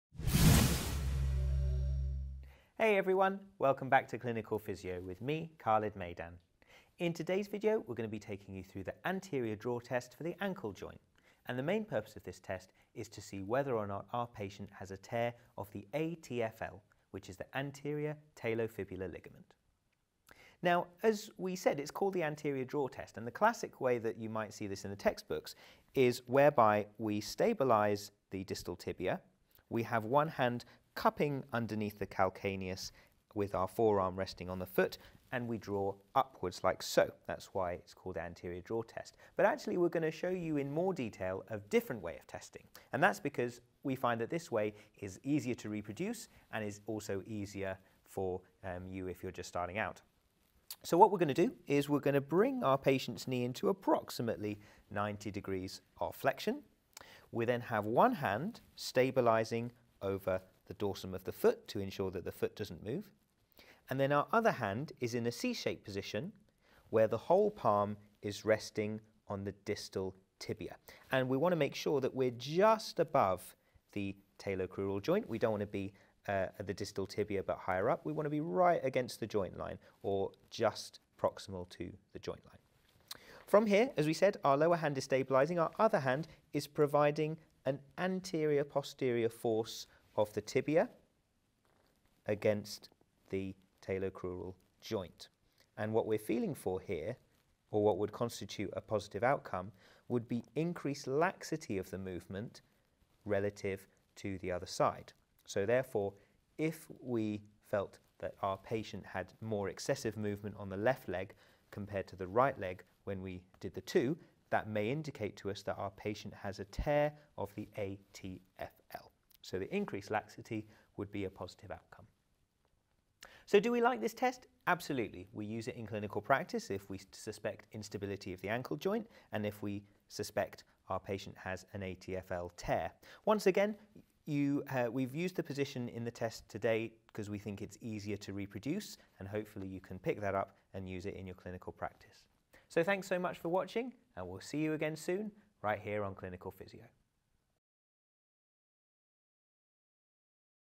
Anterior Drawer Test Ankle Ligaments | Clinical Physio Premium
In this tutorial on the anterior draw test for the ankle we will take you through everything you need to know when completing this special test. In this video we will take you through the correct handling, technique and interpretation of clinical signs.

We hope you find this video useful for your learning!

If you want to see more of these images head over to our Openstax partners